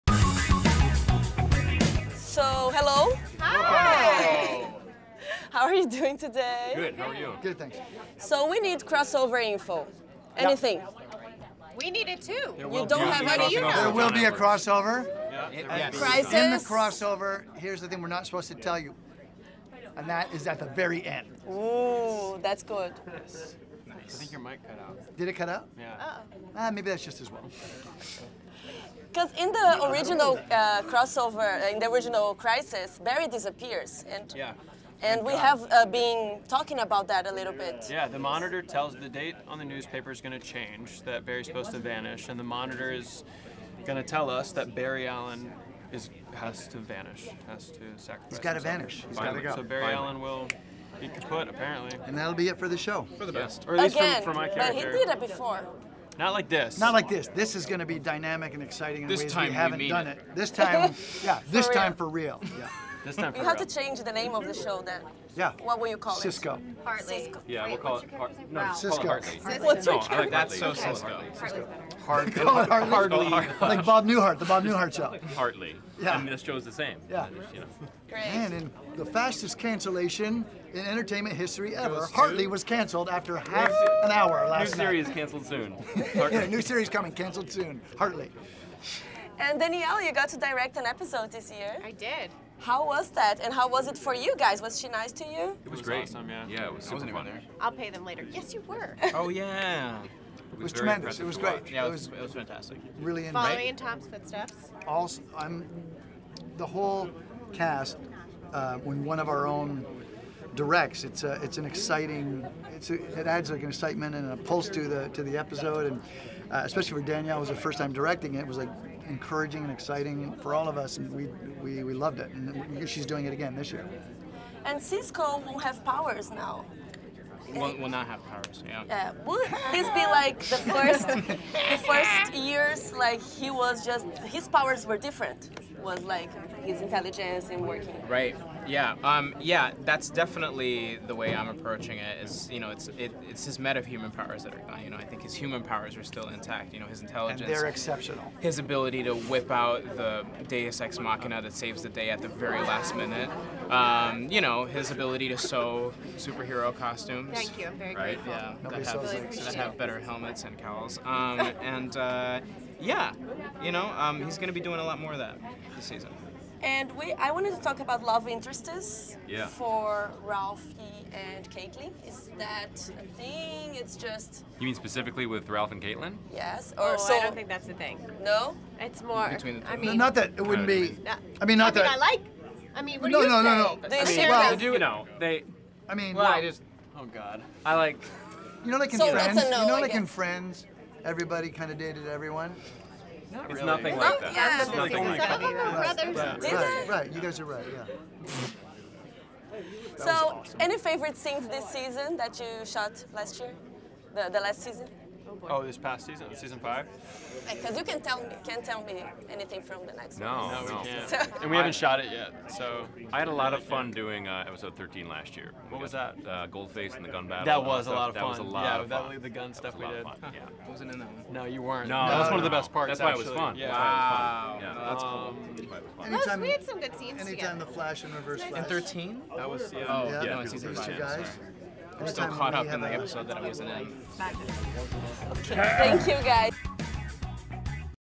ENTREVISTA FLASH | Elenco fala sobra a próxima temporada
Conversamos com exclusividade com o elenco de Flash durante a SDCC 2019 em busca de informações sobre a nova temporada da série.